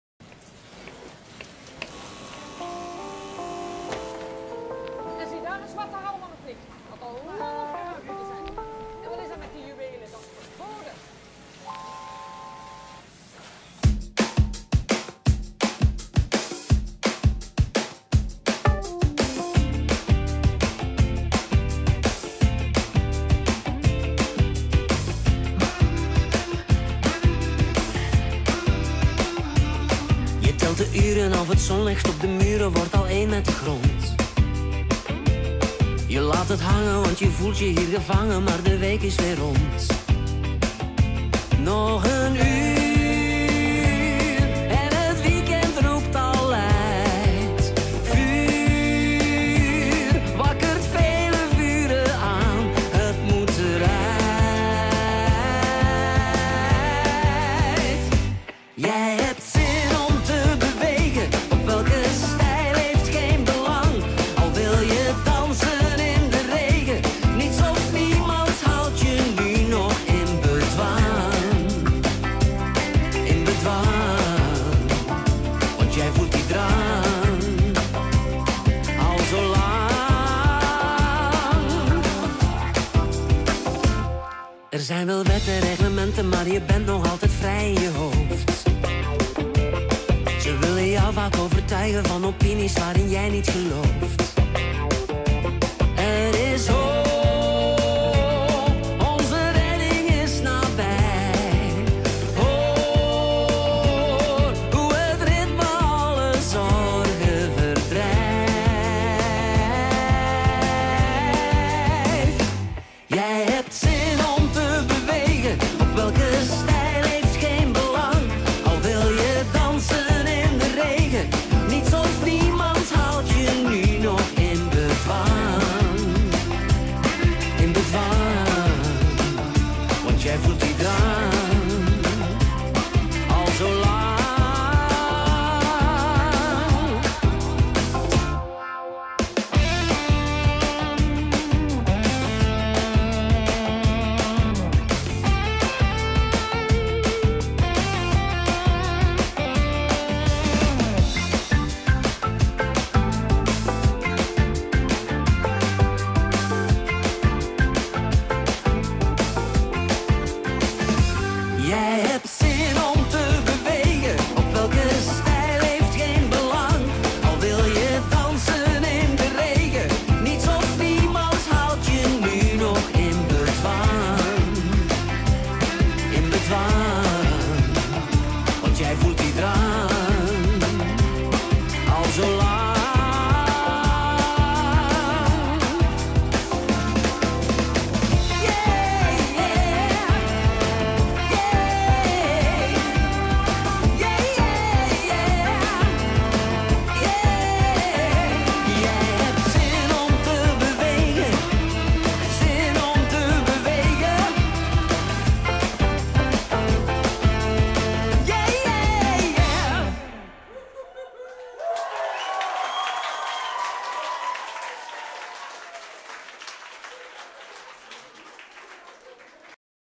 clouseau
Zonnen en huppers van de Sint-Paulus basisschool Sint-Denijs-Westrem zetten Clouseau in vuur en vlam!!!